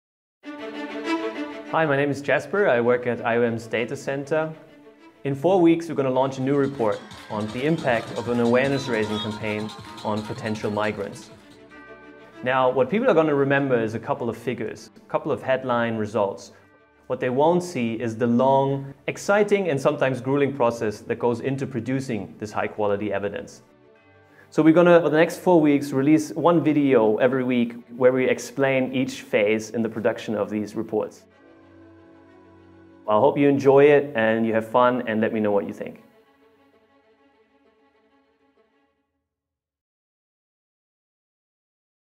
Introduction to IOM's 'Behind the Data' Series
When a report is launched, what stands out are often a few headline figures.
What remains hidden is the long, and sometimes grueling process of producing high-quality evidence.
In this mini-series, @GMDAC’s impact evaluation team explains four stages in the production of an impact evaluation report. GMDAC’s Jasper Tjaden introduces the series in this trailer.